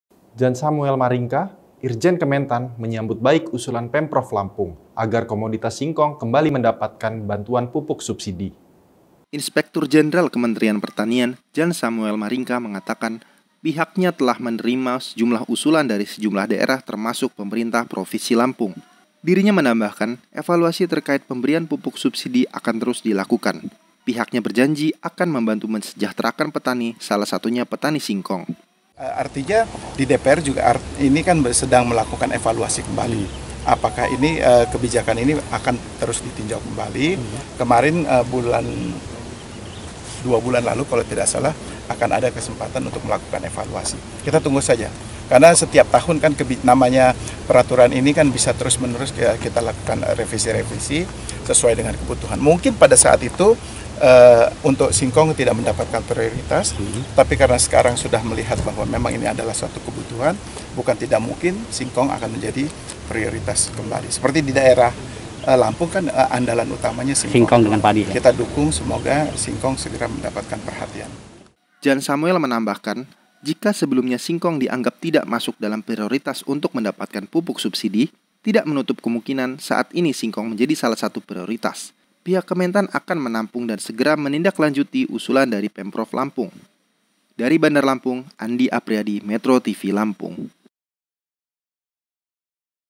Kementan Sambut Usulan Pemprov Kembali Hadirkan Pupuk Subsidi Bagi Petani Singkong
Jan Samuel Maringka, Irjen Kementan menyambut baik usulan Pemprov Lampung, agar komoditas  singkong kembali mendapat bantuan pupuk subsidi.